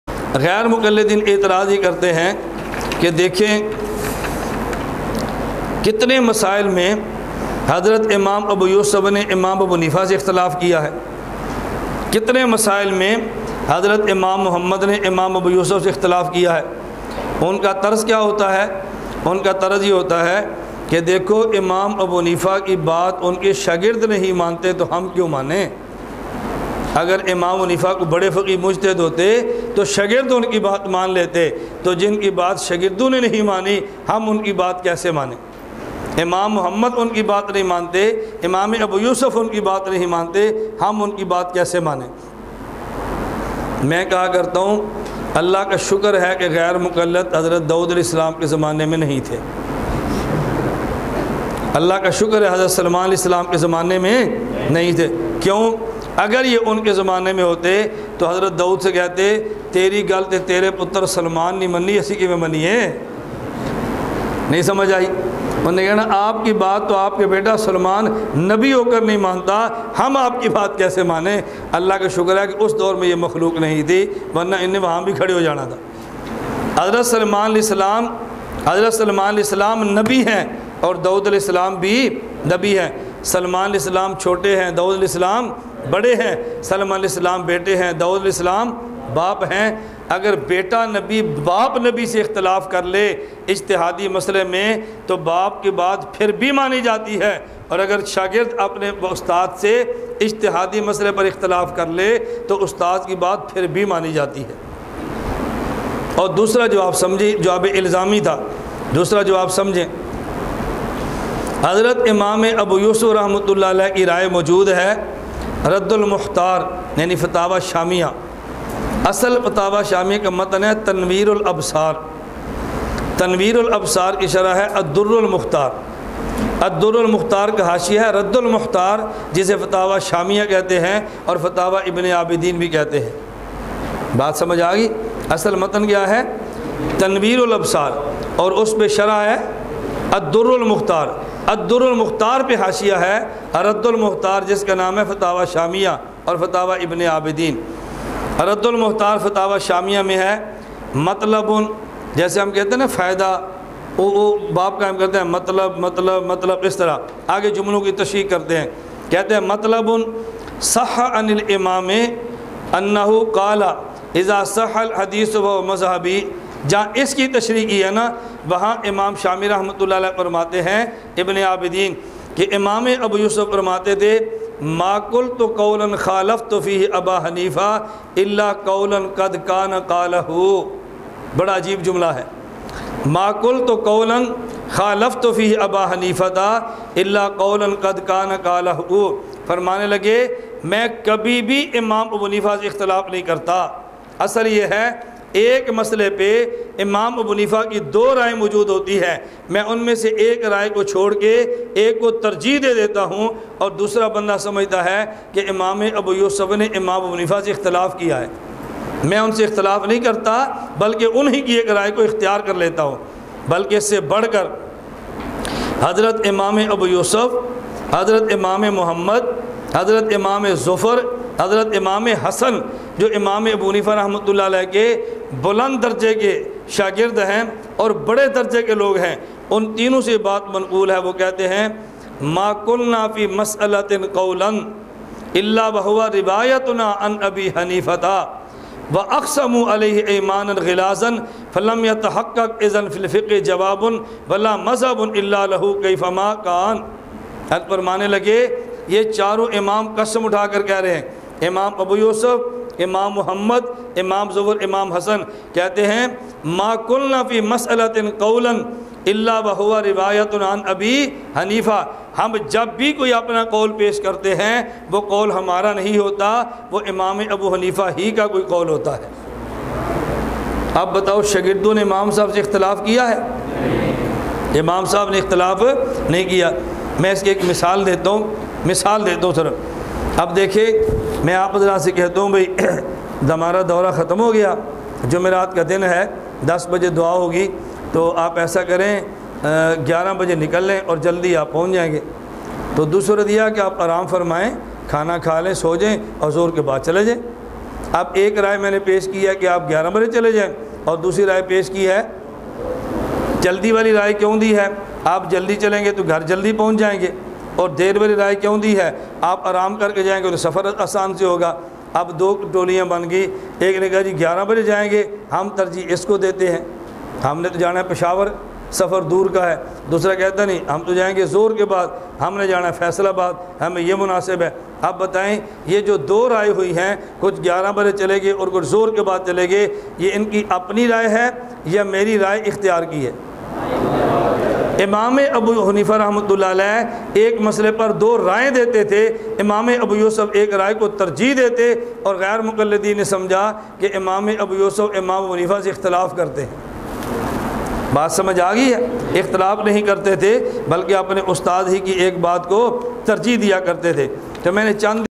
Imam AbuHanifa Ki Bat Or Un Ke Shagird, Molana Ilyas Ghuman
امام اعظم ابوحنیفہ رحمہ اللہ کی بات اور ان کے شاگرد
ایک اعتراض اور اسکا جواب
بیان : متکلم اسلام حضرت مولانا محمد الیاس گھمن دامت برکاتہم العالیہ، امیر مرکزیہ عالمی اتحاد اہلسنت والجماعت